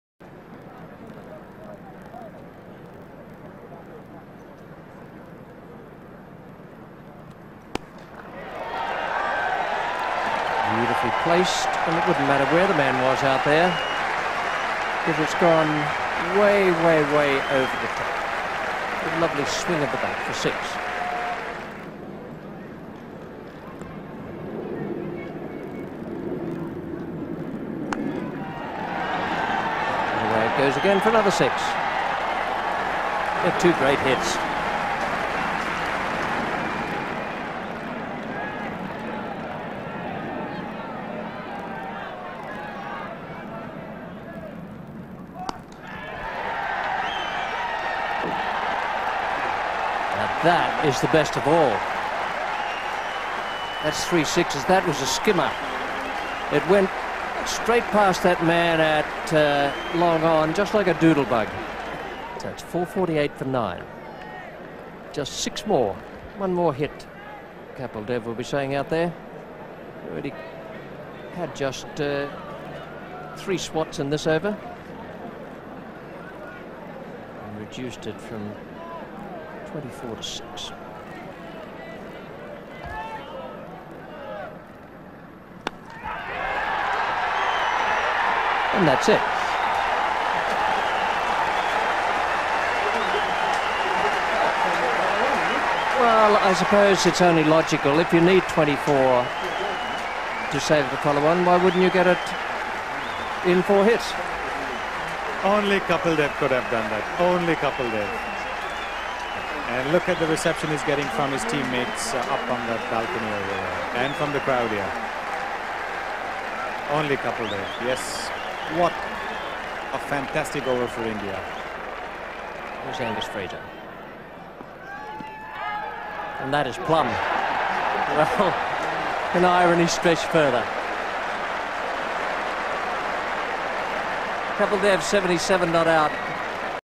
Kapil Dev hit an astonishing 4 consecutive sixes to ensure that India avoided a follow-on.
With India still needing 24 to avoid the follow-on, Dev took the initiative to wipe off the runs in the following over. After blocking the first two balls from off-spinner Eddie Hemmings, the all-rounder danced down the track and cleared the long on boundary for his first six. The same sequence followed the next delivery. Hemmings switched to a fuller length, the 31-year-old produced a flatter hit to clear the fence. The next ball was pushed wider, but the former captain stretched to get underneath the ball and connected clean for his fourth, and the visitor’s balcony applauded in delight.